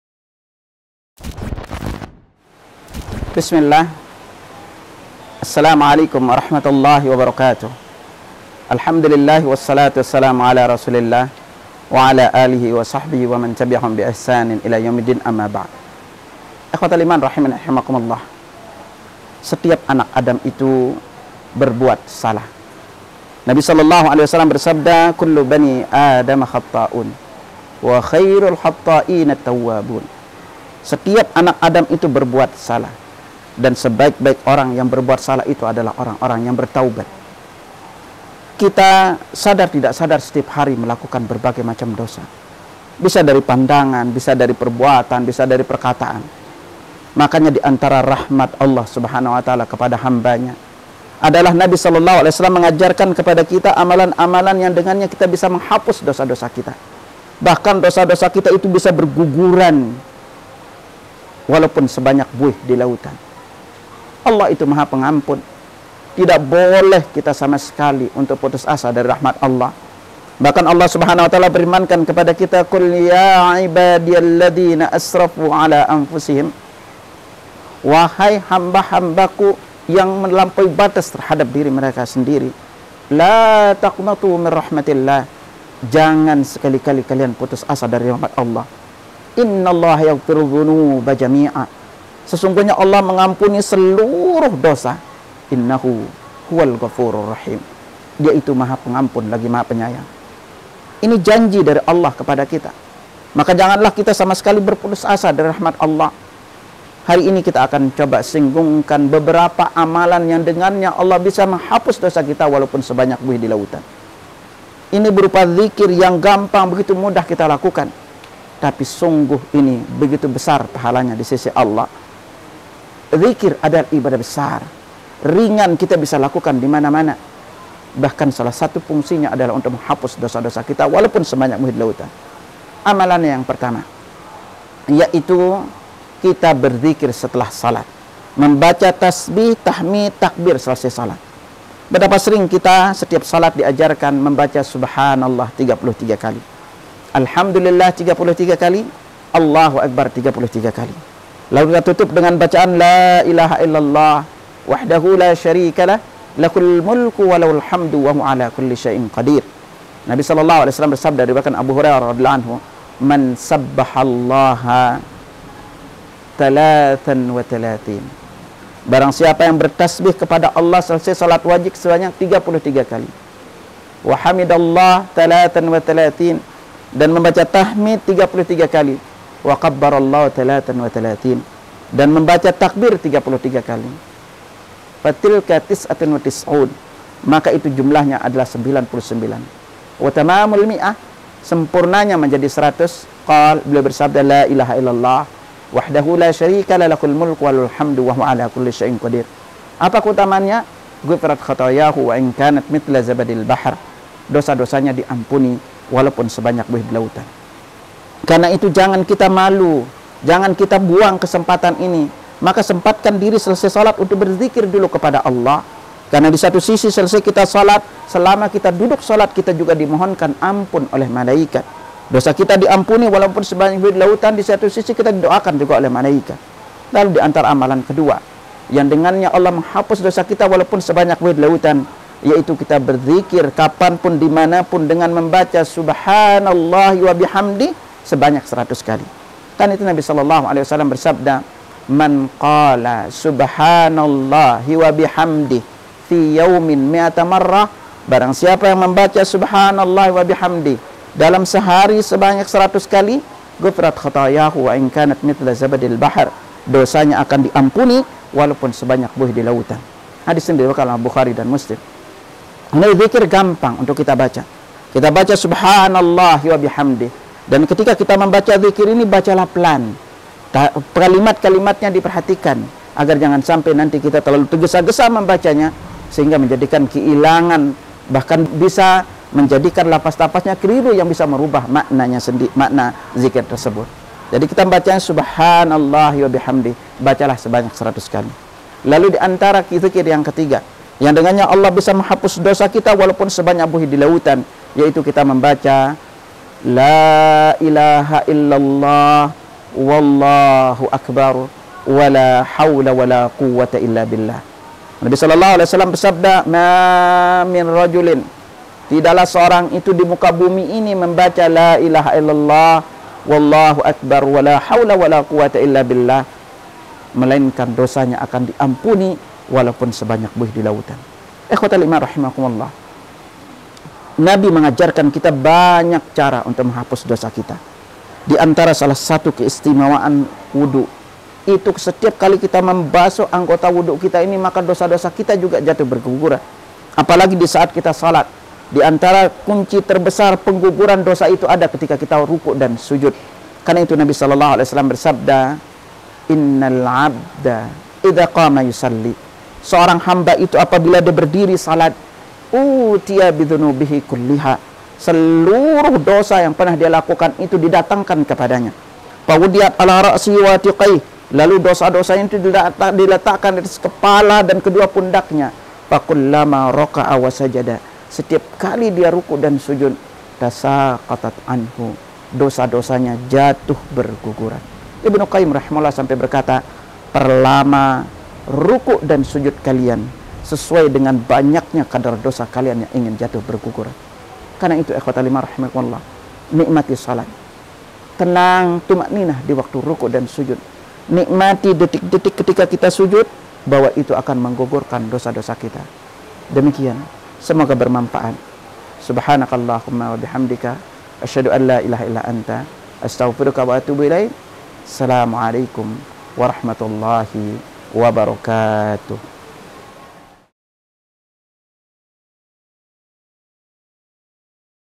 Amalan Penghapus Dosa :: Ustadz Abu Nida Lc,. - CKSTV
Beberapa amalan yang bisa menghapuskan dosa walau dosanya sebanyak buih di lautan.

simak penjelasannya di video ini